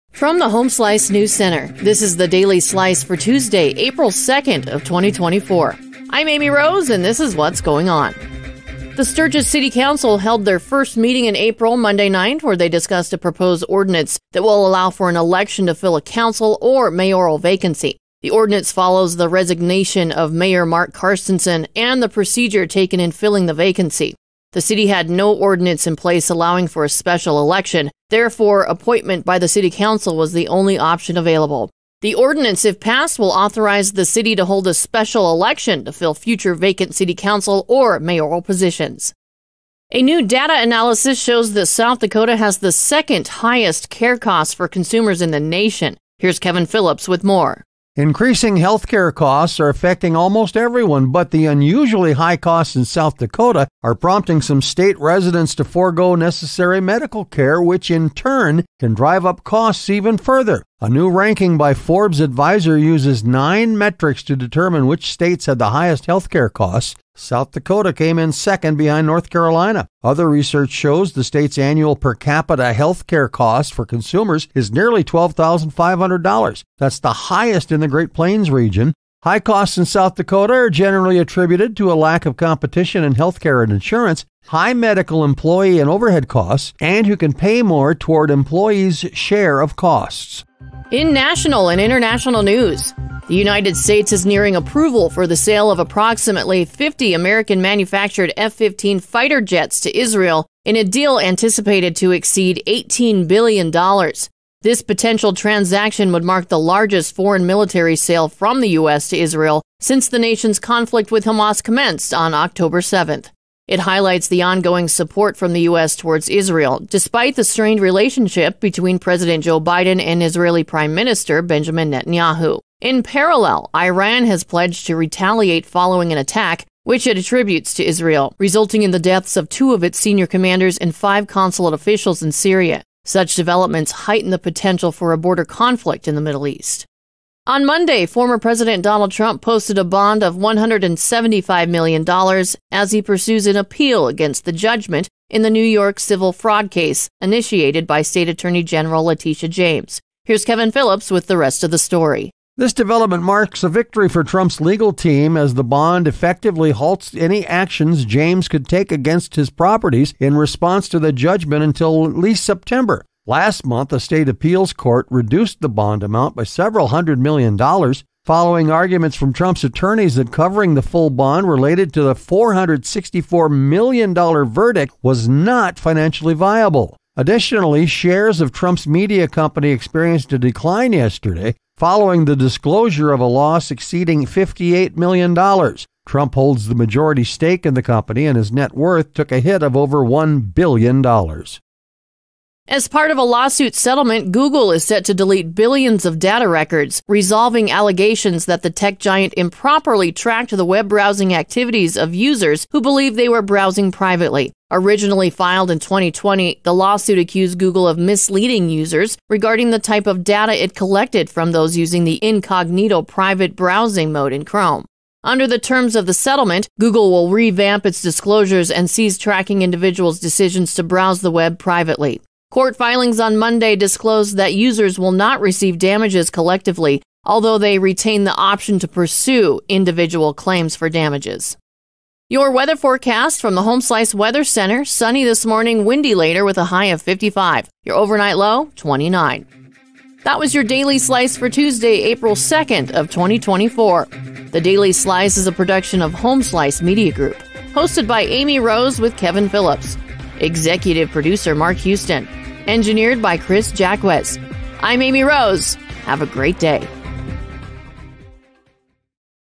Daily Slice for Tuesday, April 02, 2024
The Sturgis City Council discussed a proposed ordinance that will allow for an election to fill a council or mayoral vacancyA new data analysis shows that South Dakota has the second-highest health care costs for consumers in the nationThe United States is nearing approval for the sale of approximately 50 American-manufactured F-15 fighter jets to Israel Donald Trump posted a bond of $175 million as he pursues an appeal against the judgment in the New York civil fraud case Google is set to delete billions of data records, resolving allegations that the tech giant improperly tracked the web-browsing activitiesWeather